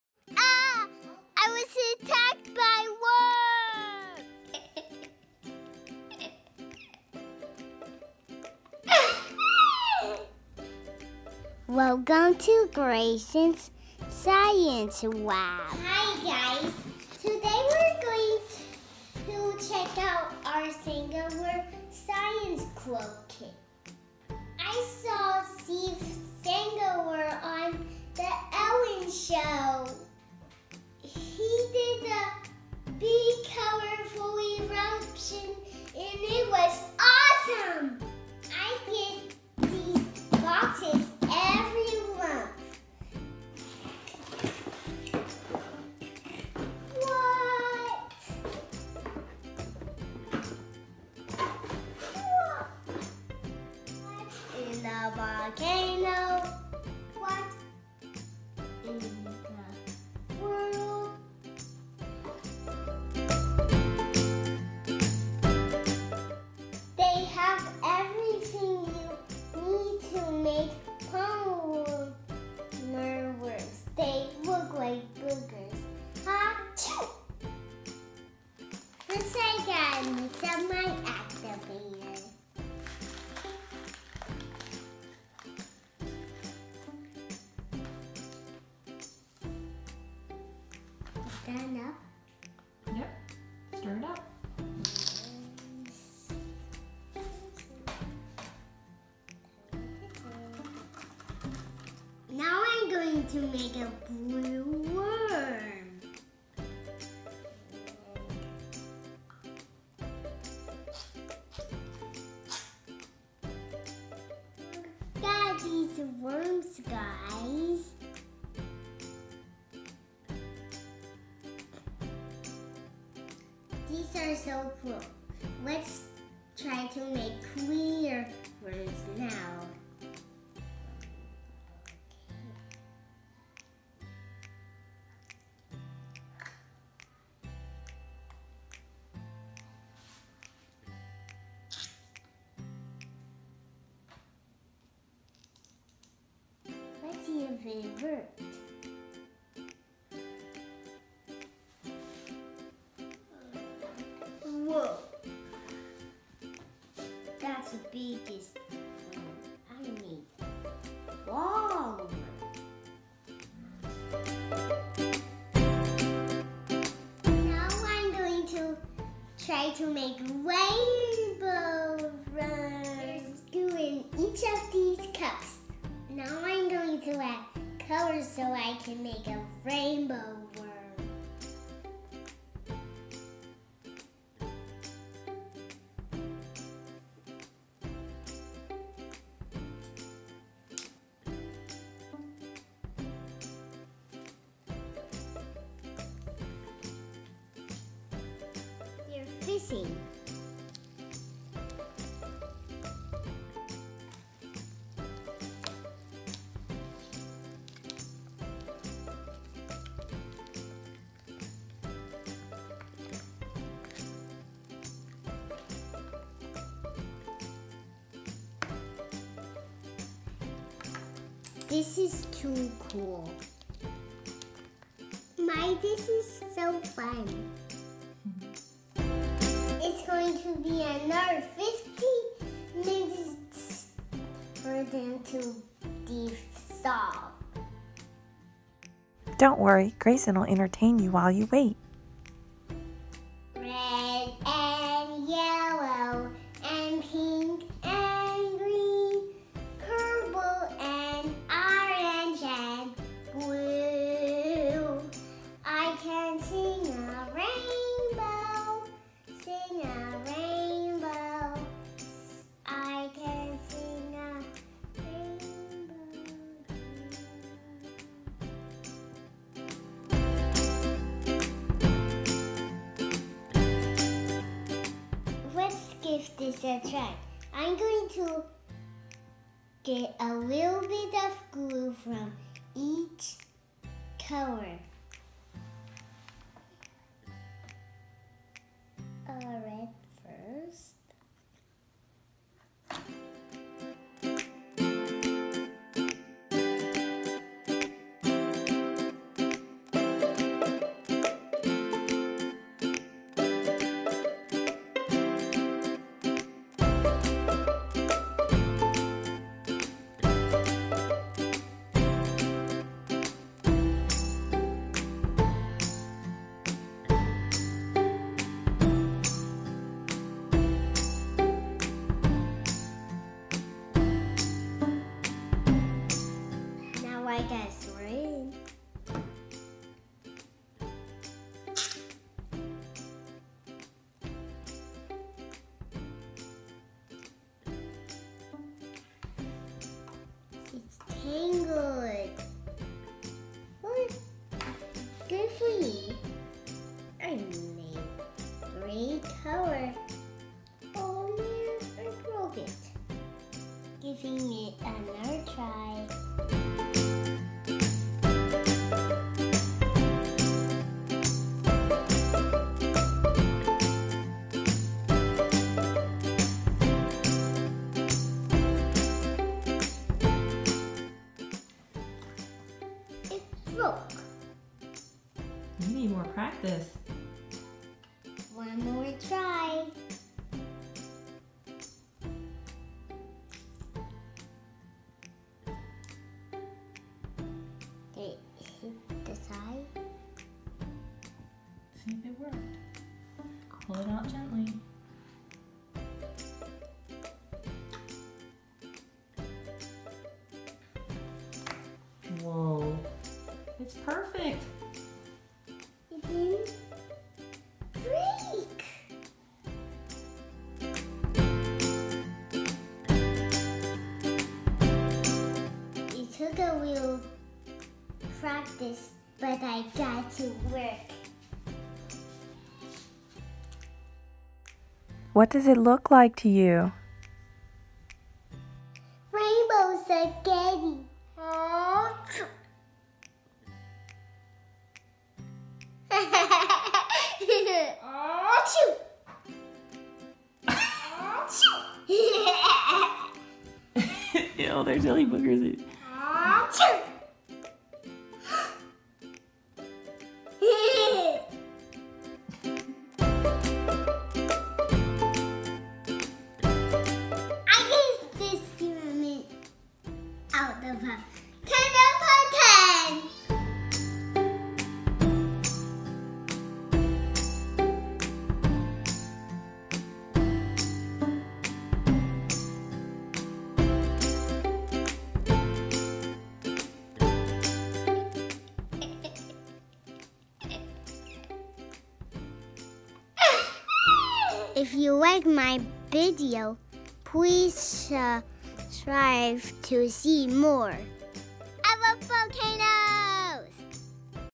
Steve Spangler Science Club August Box Review by a Kid
Gracyn reviews the Spangler Science Club's August box and learns about polymer worms! Watch Gracyn as she demonstrates how to make slimey gooey rainbow worms!

Here's the link to the huge experiment Gracyn loves that was shown on the Ellen Show

Shop Gracyn's Science Kits on Facebook